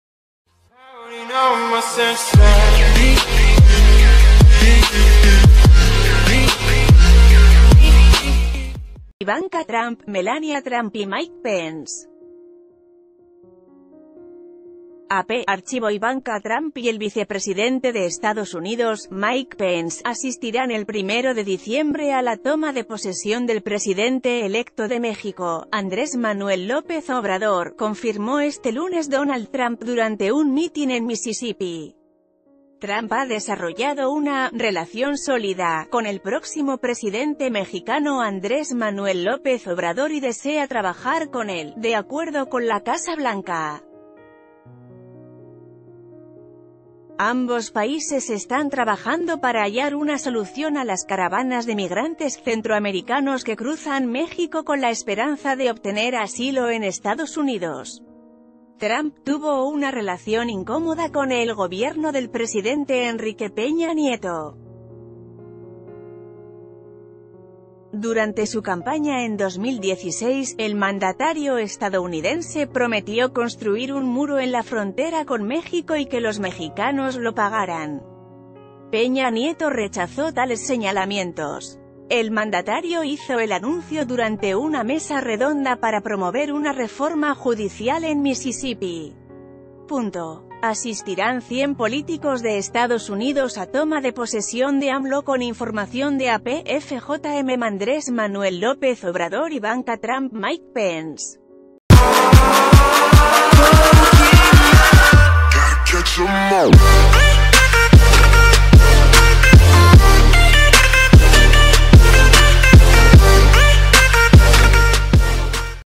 Ivanka Trump y Mike Pence asistirán a toma de posesión de López Obrador
Ivanka Trump, Melania Trump y Mike Pence. AP/ArchivoIvanka Trump y el vicepresidente de Estados Unidos, Mike Pence, asistirán el 1 de diciembre a la toma de posesión del presidente electo de México, Andrés Manuel López Obrador, confirmó este lunes Donald Trump durante un mitin en Mississippi. Trump ...